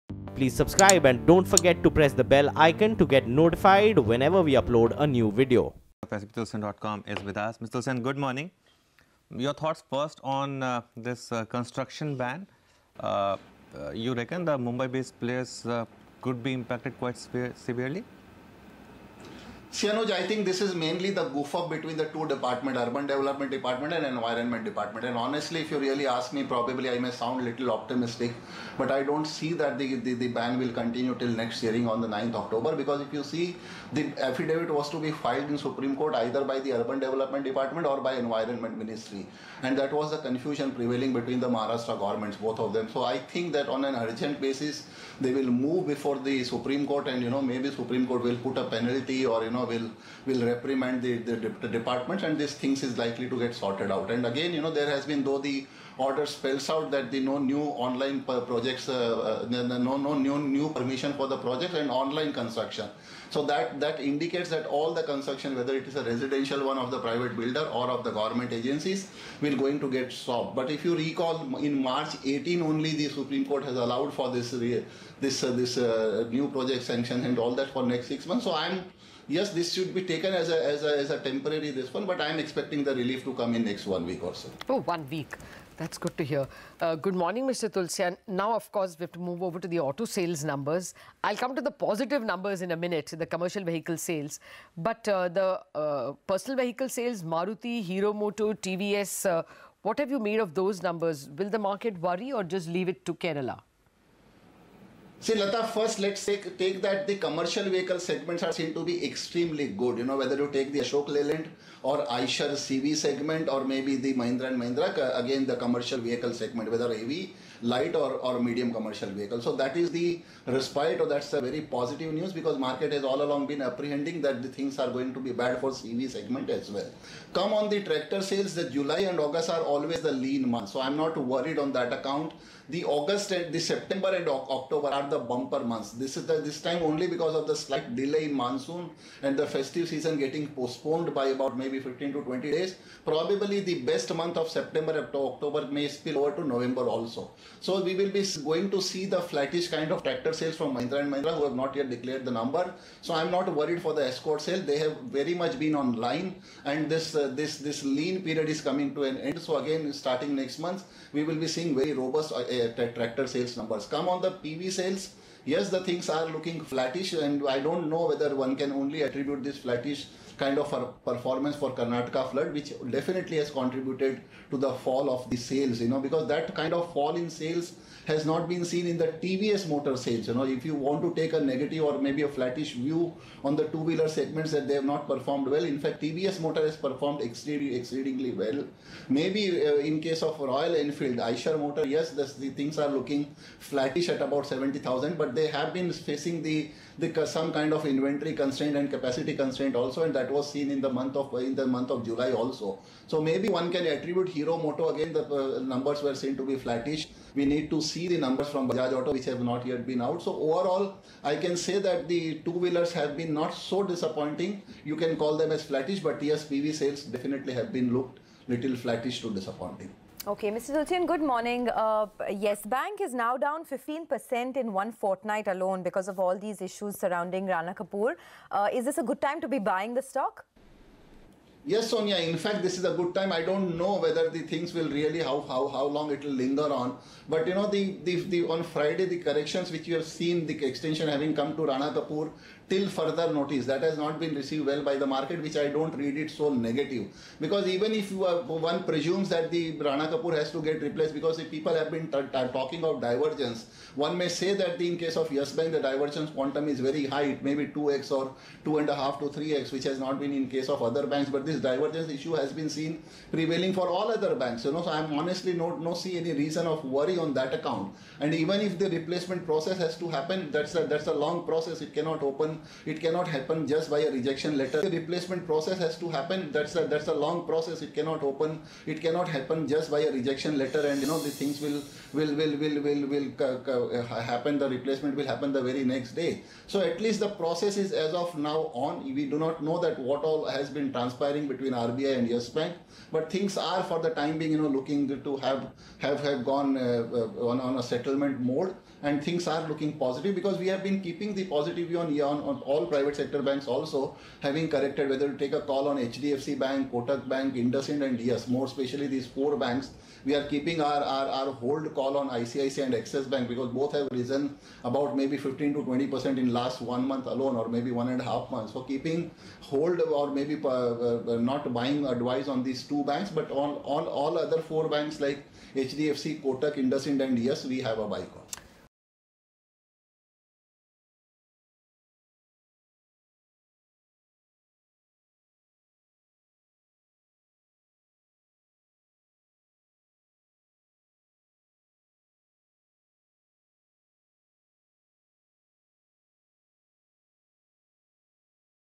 SP Tulsian on Construction Ban in Maharashtra, Madhya Pradesh, Uttarakhand | CNBC TV18
SP Tulsian's take on Real estate shares which are trading lower by 2-3% after the Supreme Court (SC) banned further construction in a few states.